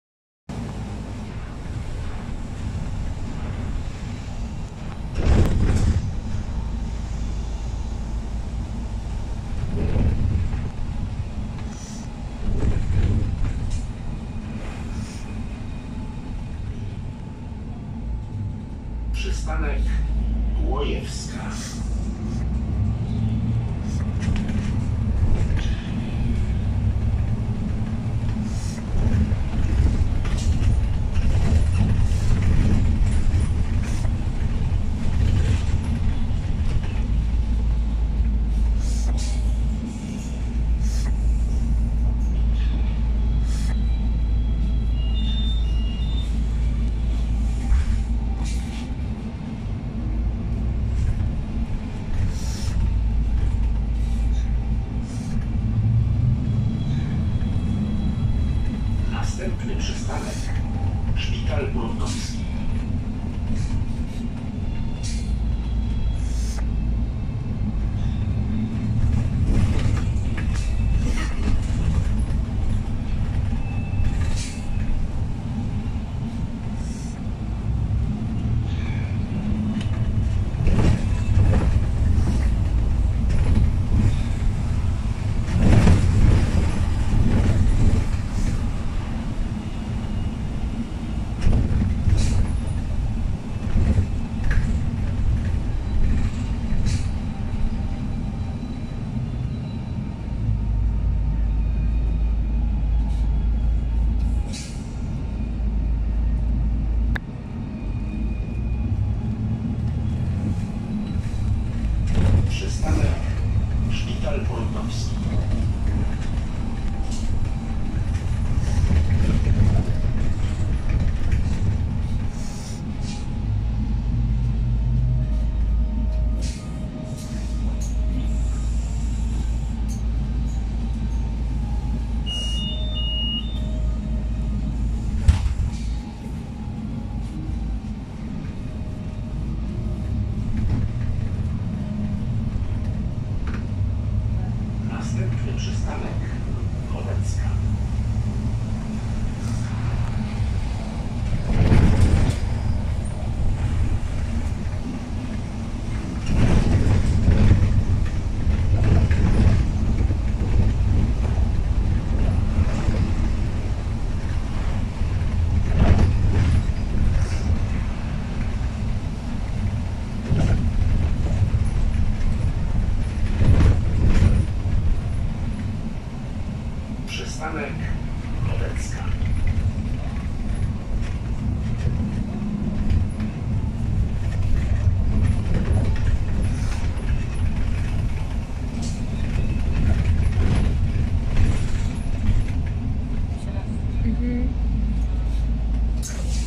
Przejazd solarisem urbino 18 linia 169 w kierunku Chodecka #8857
Cześć dziś się przejedziemy solarisem urbino 18 linia 169 w kierunku Chodecka